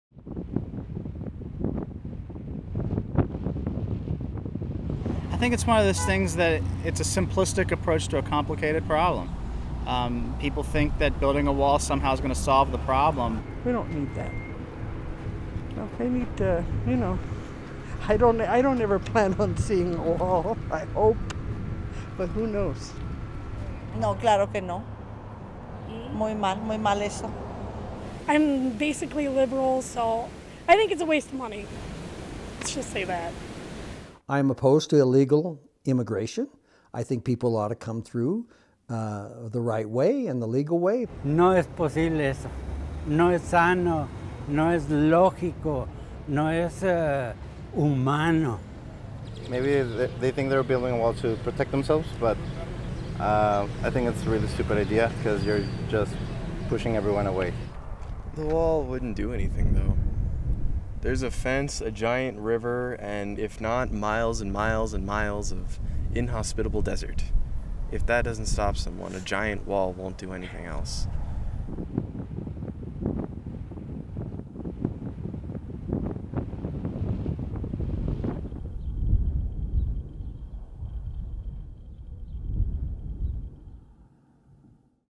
What U.S.-Mexico border residents think of building a wall | Cronkite News
The Cronkite News-Univision News-Dallas Morning News poll of 1,427 residents living along the U.S.-Mexico border found that majorities in cities on both sides oppose building a wall along the border.

The poll was conducted by Baselice & Associates Inc., and has a margin of error of +/- 3.7 percentage points.

(Video by Courtney Pedroza/Cronkite News)